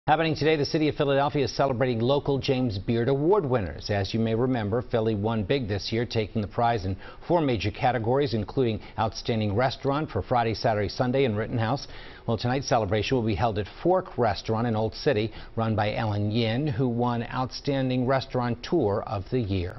Philadelphia celebrates local James Beard award winners
Philly won big this year, taking the prize in four major categories, including "outstanding restaurant" for "Friday, Saturday, Sunday" in Rittenhouse.